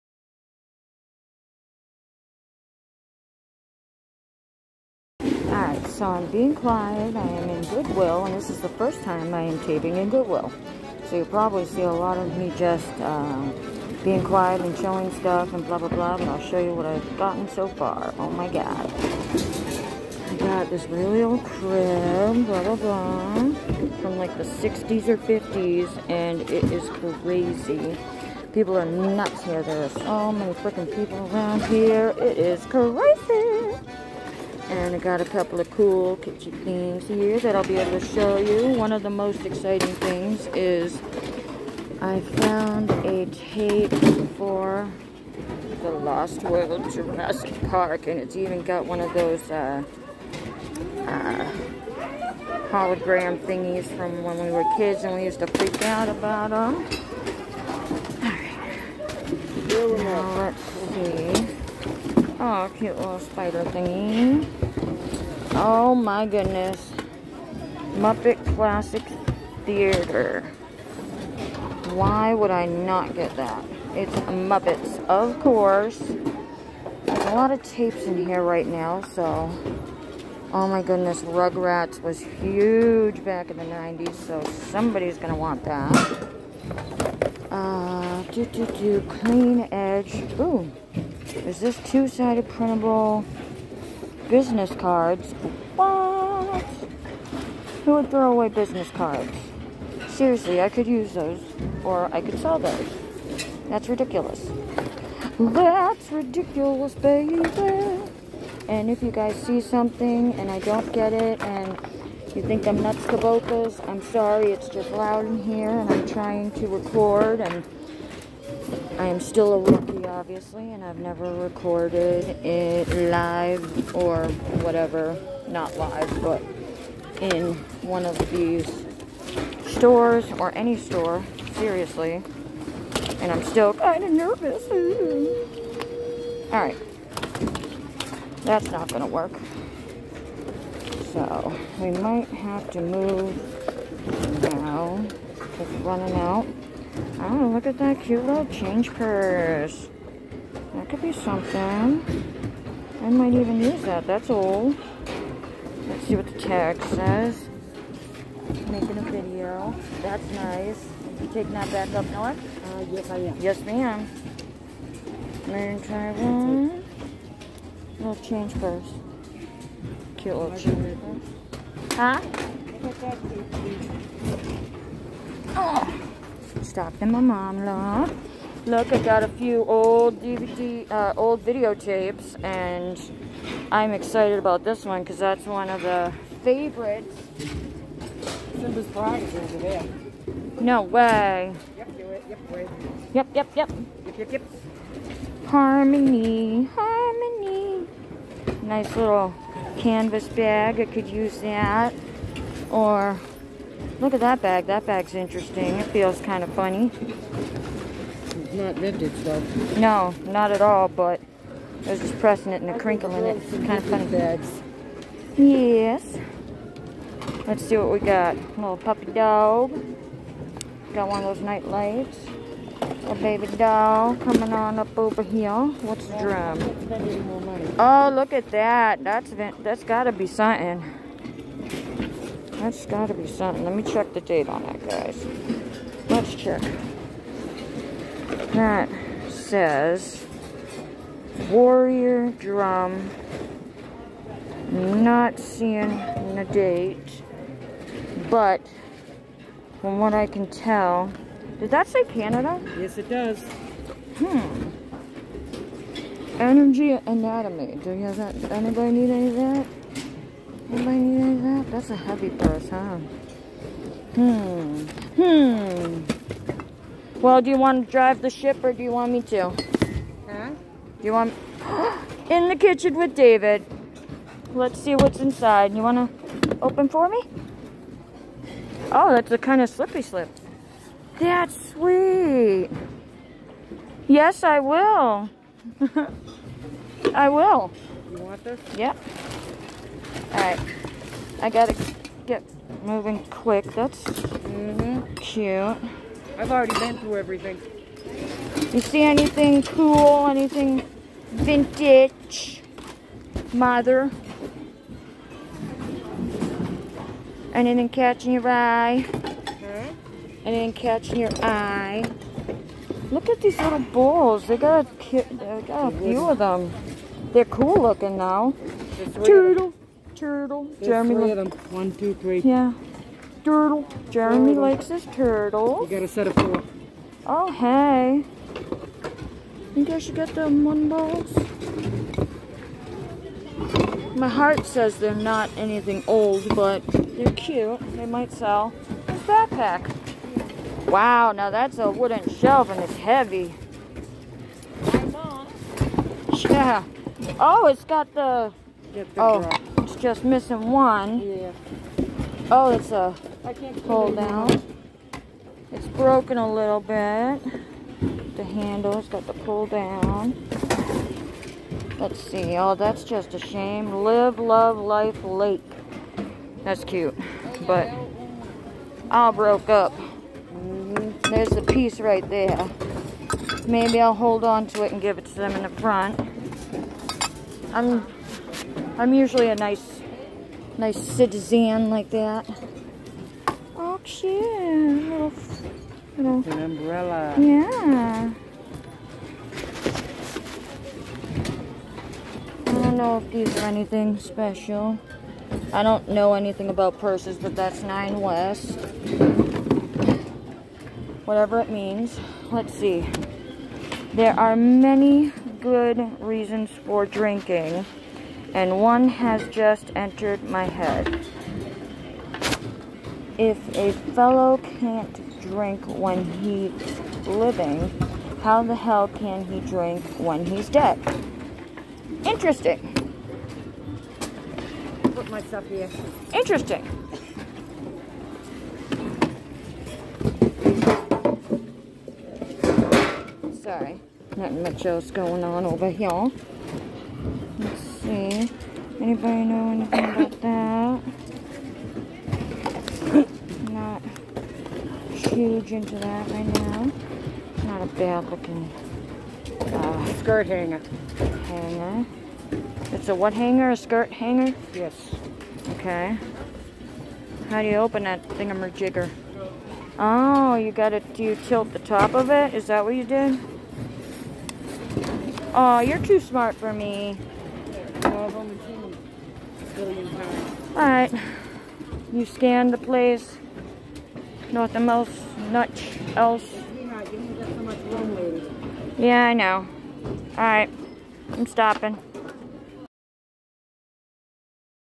Quick stop at my local Goodwill 😊
Don't mind me guys, I was just out and about with the Mother in Law on Saturday and decided to make my debut shopping video. Went to my local Goodwill and picked up a few amazing treasures, like a 1960s baby crib metal toy worth a couple hundred and I paid 99 cents! Woop! short and sweet enjoy.

AND DONT FORGET TO👍:yt:🔔and SHARE!!!💥Just do it, it's free!💥

ebay- theycallnemomma86

and FB GROUP is under- Rosies Random Treasures